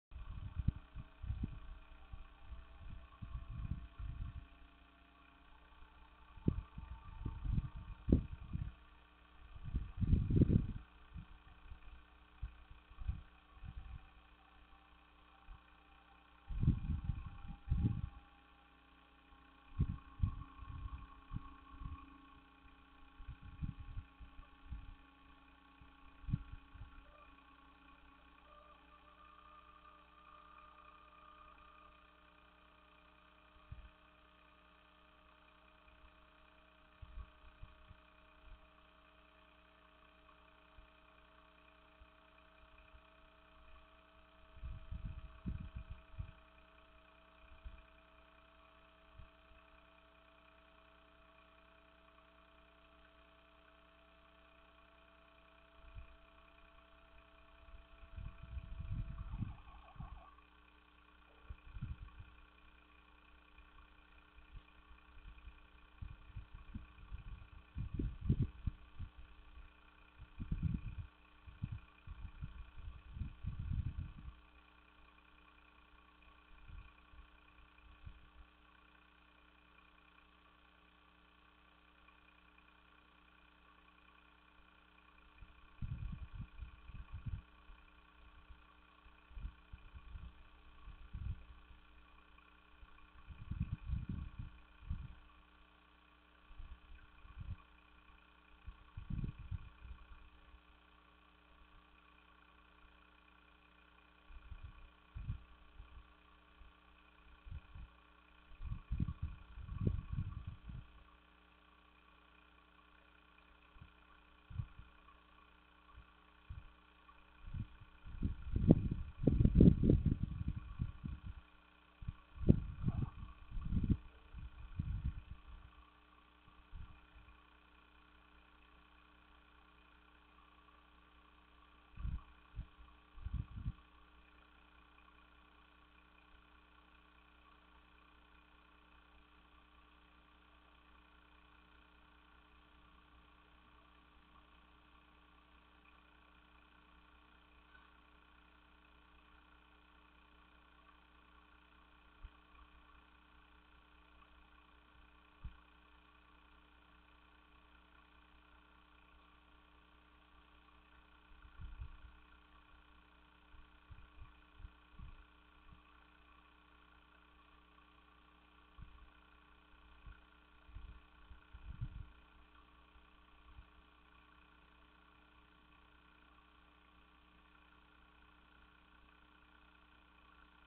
Porcupine looking for food close up.
Close up of porcupine looking for food.